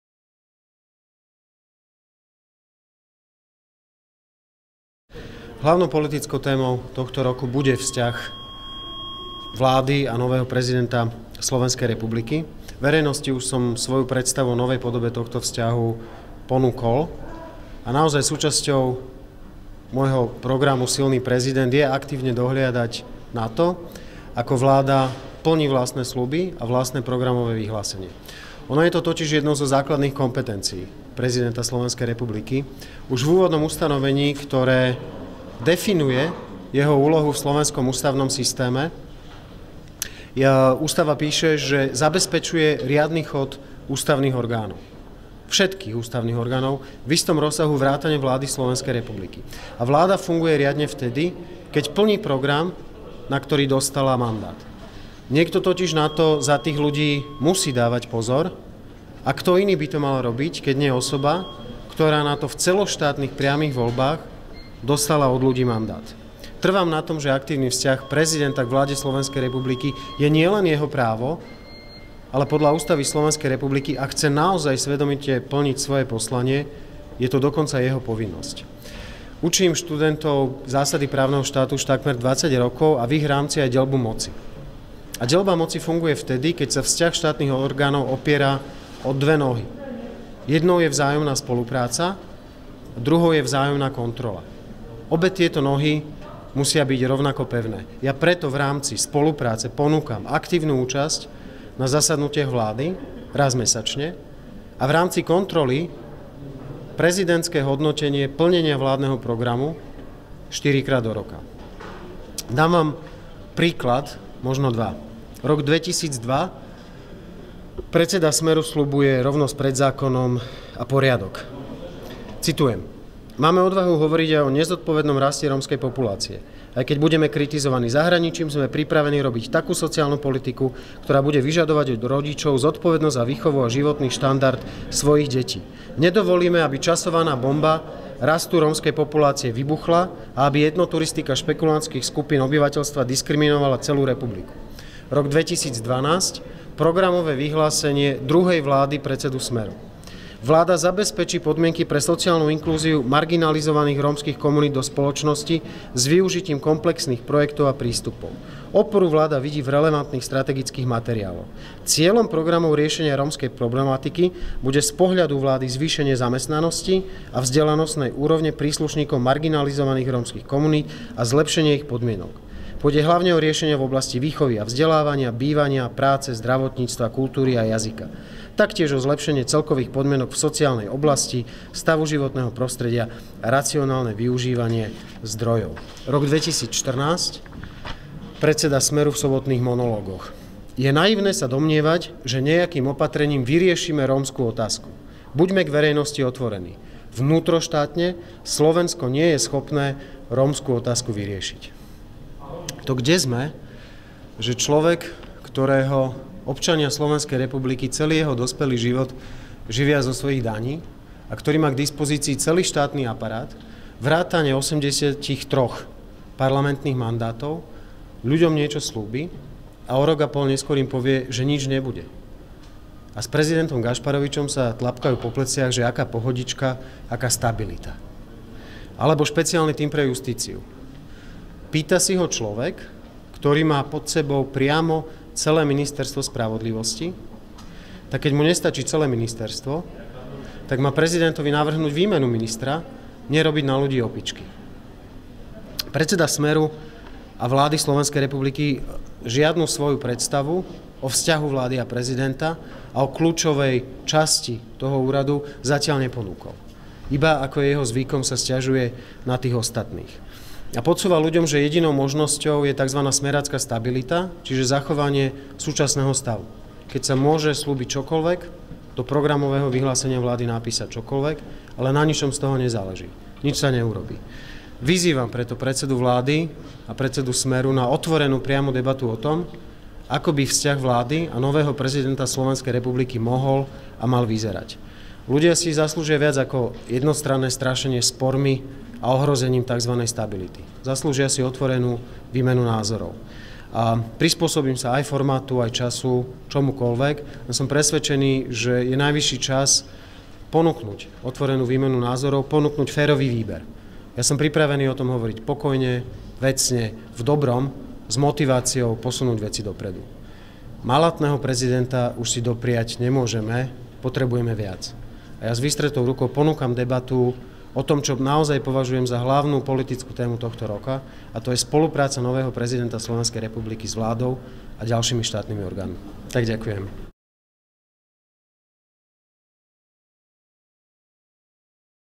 Rado Procházka vyzýva Roberta Fica na otvorenú diskusiu
Predseda Smeru a vlády Slovenskej republiky žiadnu svoju predstavu o vzťahu vlády a prezidenta a o kľúčovej časti toho úradu zatiaľ neponúkol. Iba, ako je jeho zvykom, sa sťažuje na tých ostatných. Podsúva ľuďom, že jedinou možnosťou je takzvaná "smerácka stabilita", čiže zachovanie súčasného stavu, keď sa môže sľúbiť čokoľvek, do programového vyhlásenia vlády napísať čokoľvek, ale na ničom z toho nezáleží, nič sa neurobí. Vyzývam preto predsedu vlády a predsedu Smeru na otvorenú debatu o tom, ako by vzťah vlády a nového prezidenta Slovenskej republiky mohol a mal vyzerať. Ľudia si zaslúžia viac ako jednostranné strašenie spormi a ohrozením takzvanej stability, zaslúžia si otvorenú výmenu názorov. Malátneho prezidenta si už dopriať nemôžeme, potrebujeme viac. Ja s vystretou rukou ponúkam debatu o tom, čo naozaj považujem za hlavnú politickú tému tohto roka, a to je spolupráca nového prezidenta Slovenskej republiky s vládou a ďalšími štátnymi orgánmi.